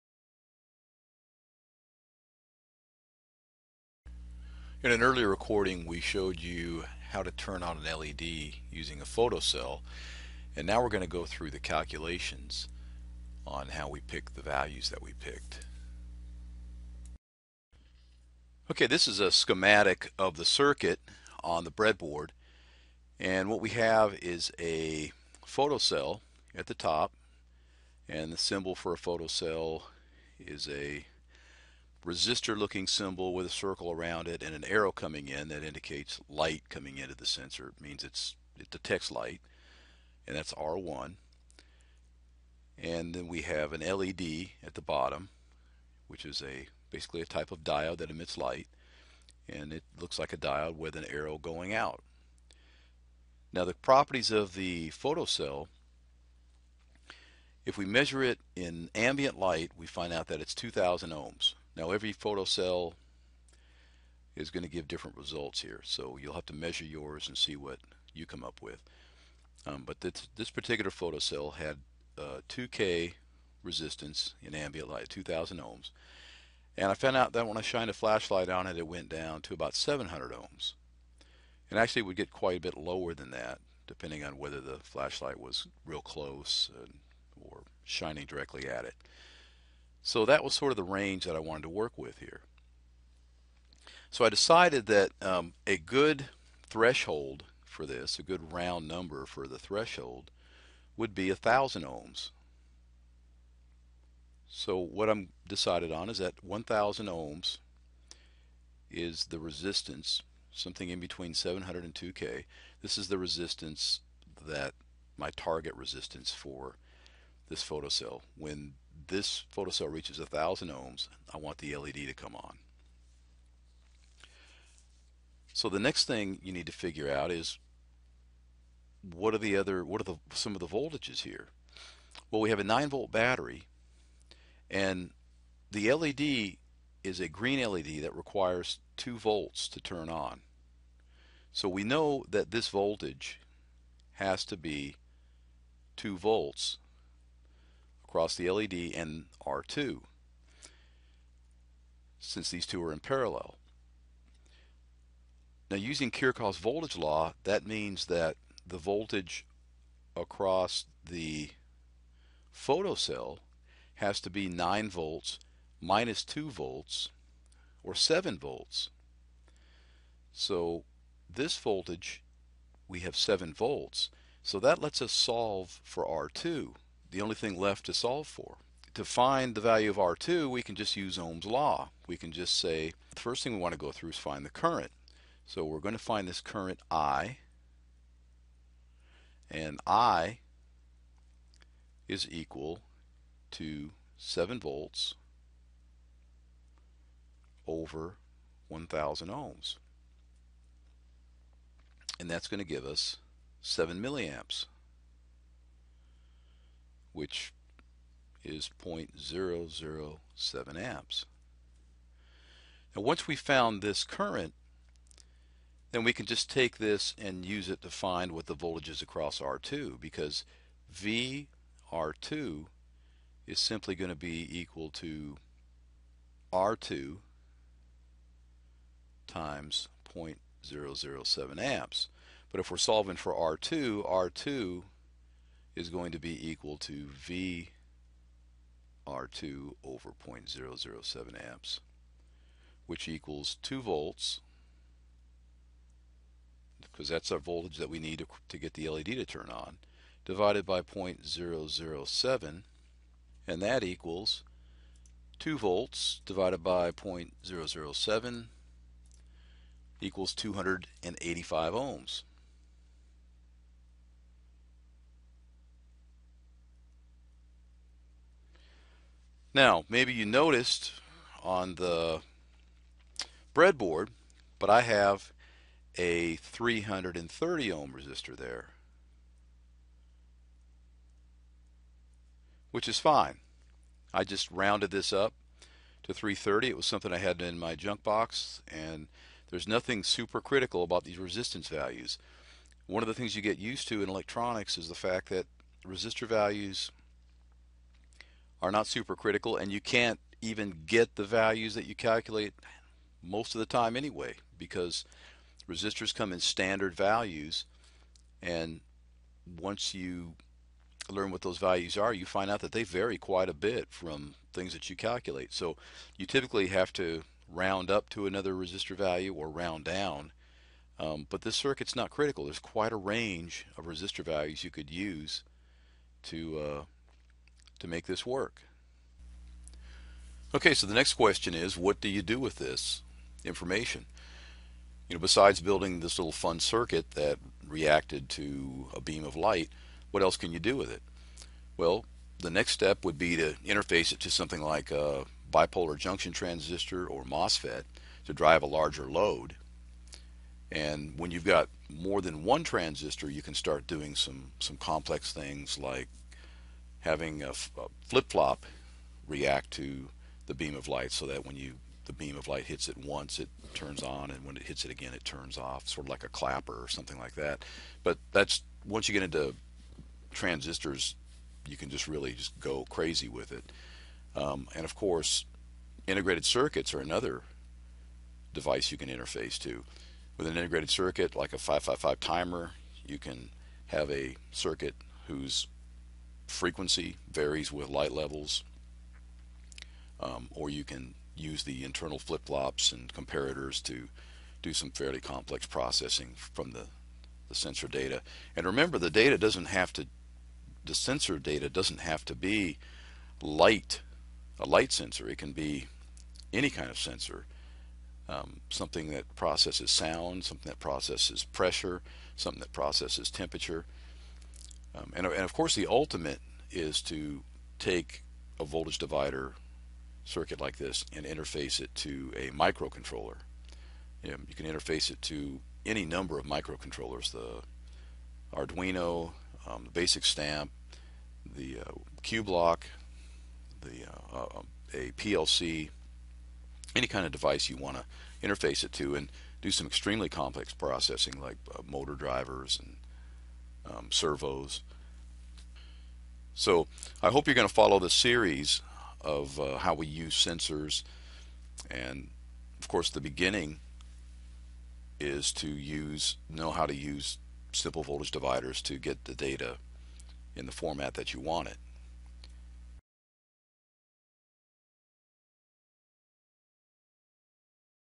Voltage Dividers, Photocells and LEDs DIY
A tutorial about using voltage dividers in sensing. An LED is driven from a photocell sensor to allow the LED to turn on with light or turn off with light. This is a great way to discuss voltage dividers with students while doing something interesting.

If the photocell resistance decreases with light and increases with darkness we show how to calculate values of resistance and configure the circuit to turn on the LED with light and turn off the LED with light.

The circuit uses only three simple components to perform a fairly complicated and interesting task.